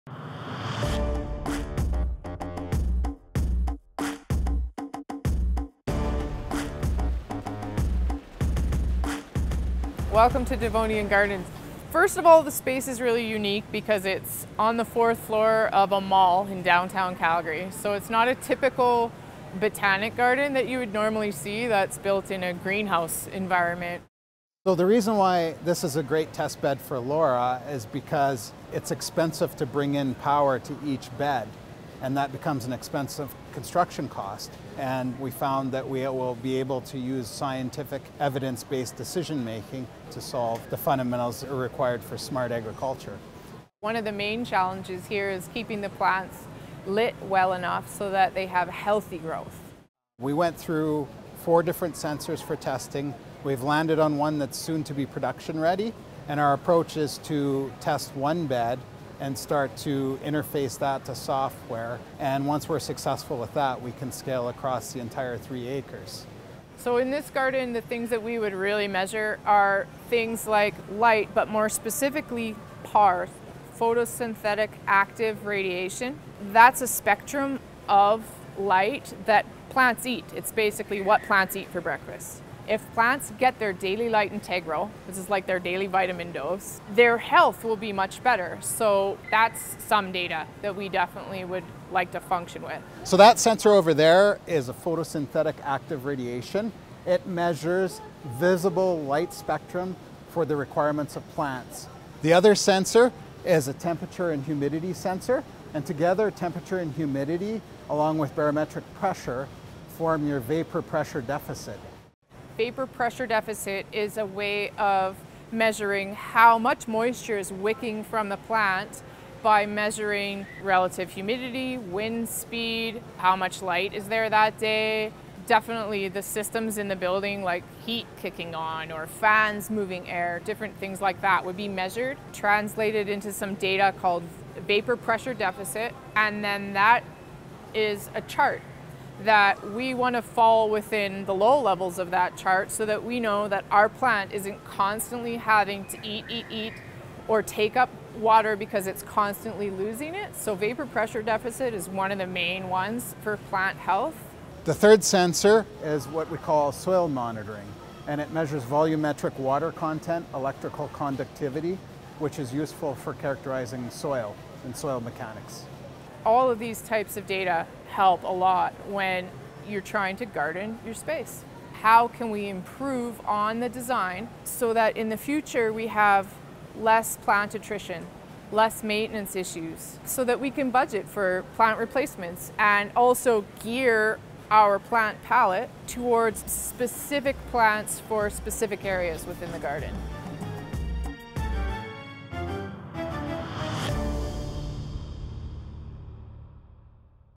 Smart Calgary Use Case: Devonian Gardens
Devonian Gardens, an indoor botanical garden in the City of Calgary, leverages LoRa Technology in smart agriculture applications to optimize garden health.